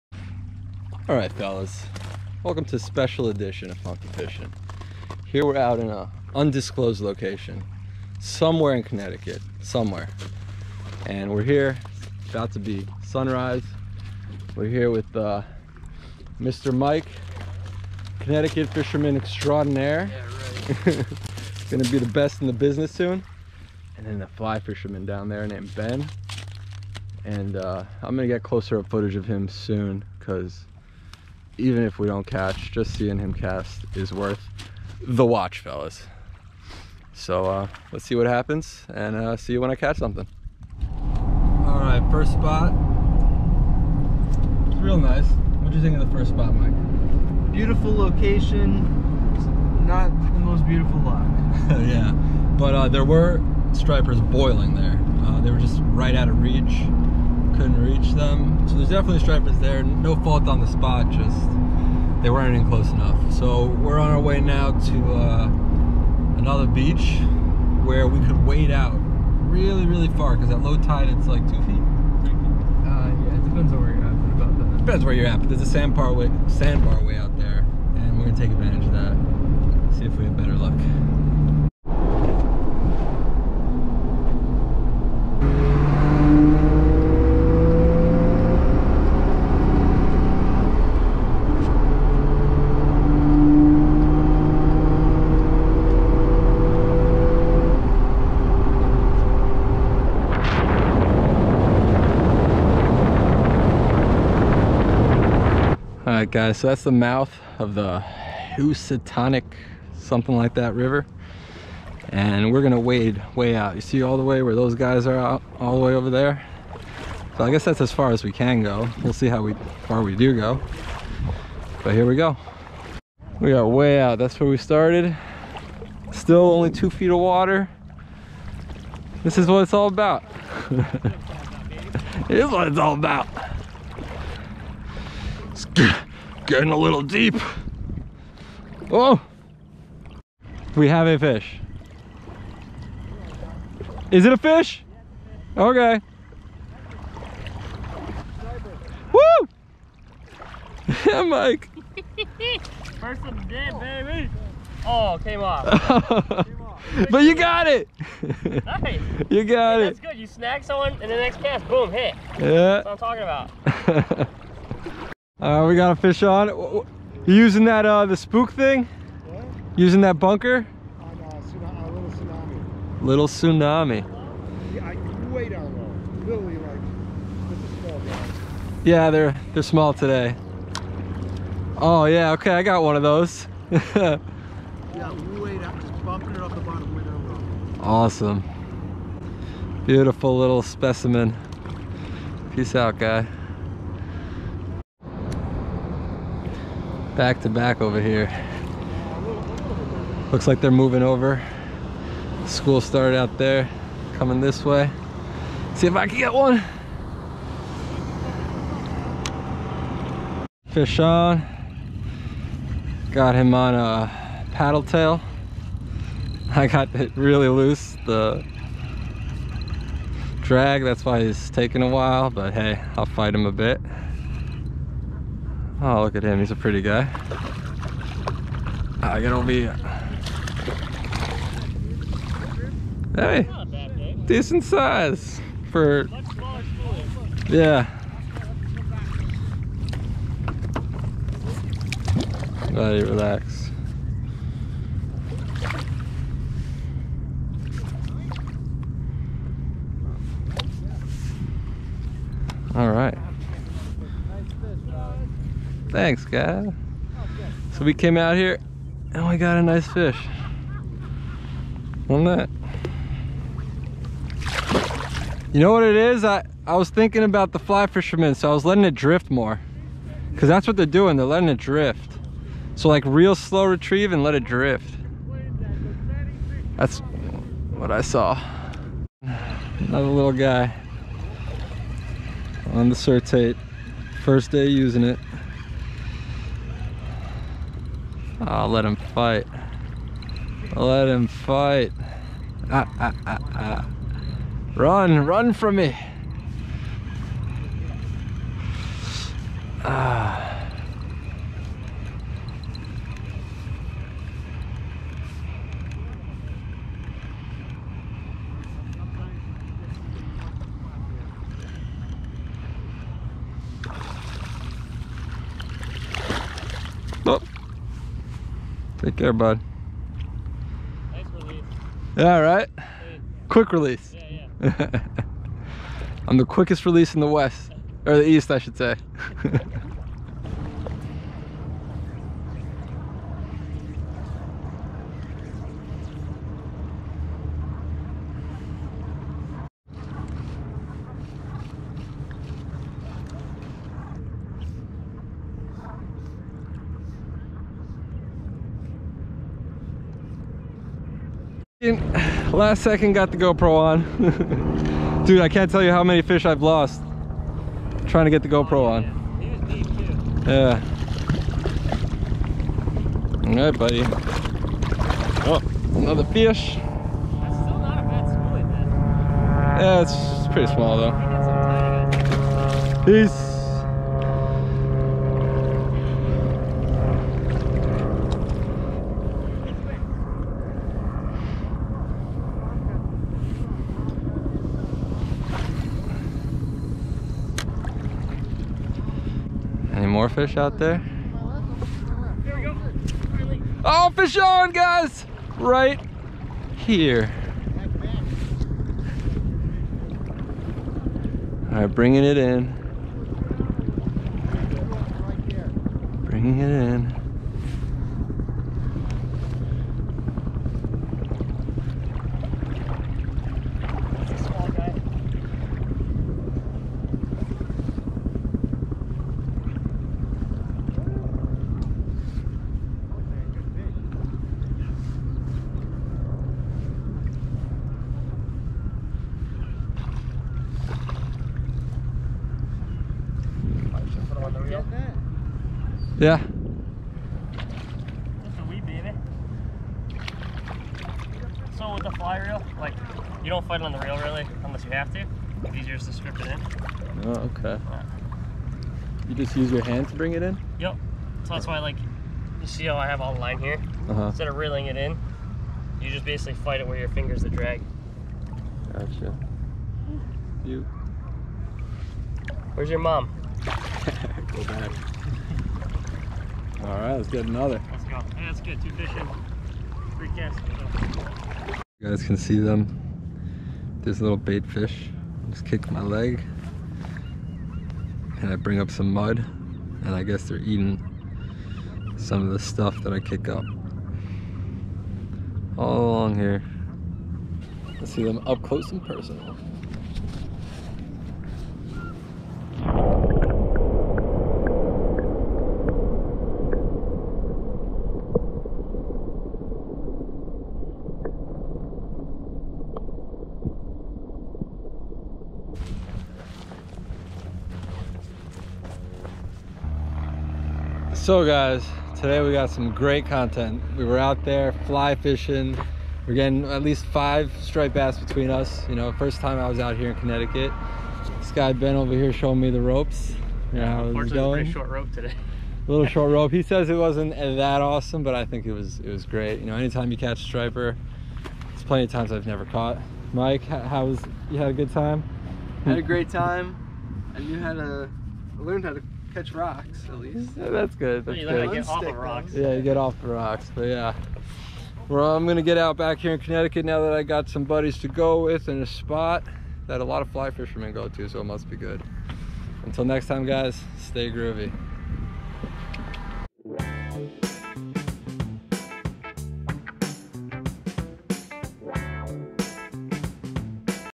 Fly Fishing For STRIPED BASS: Stratford Connecticut
Today we meet up with my friend Mike and Ben in Connecticut to catch some stripers. Ben is a fly fisherman and showed us the basics of fly fishing in saltwater. We manage get into a couple of schoolies at the mouth of the Housatonic River and what a Great Day of wading for stripers!!!